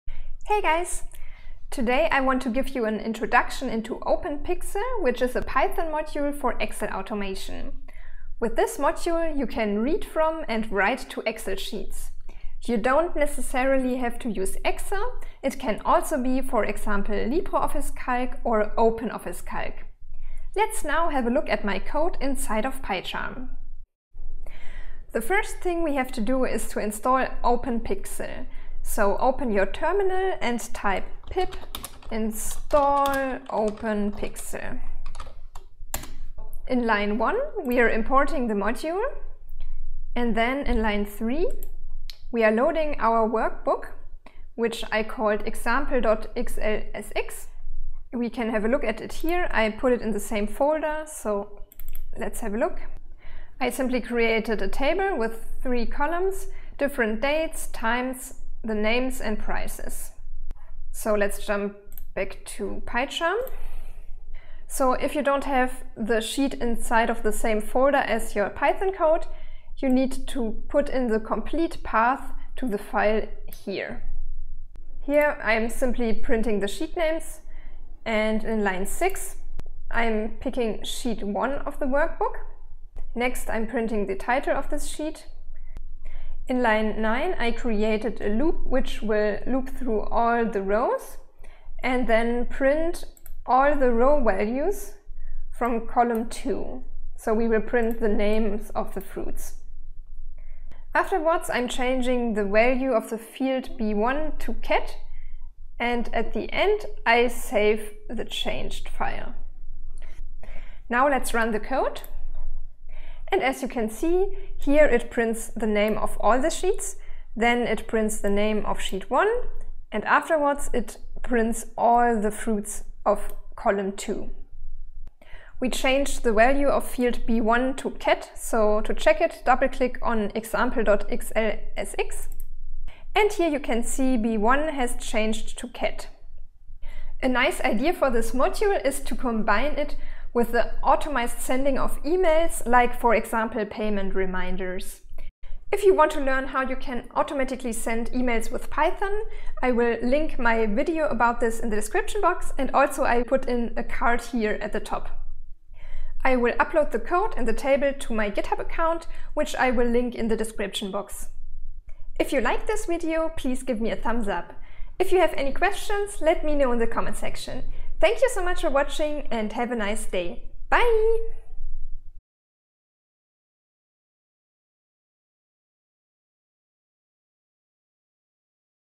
Excel automation with Python and Openpyxl
Hey guys! Today Im showing you an introduction about how you can use the Python module Openpyxl for Excel automation. What was the last thing you automated? Have a nice day. :)

➤ "Automate the boring stuff with Python" book: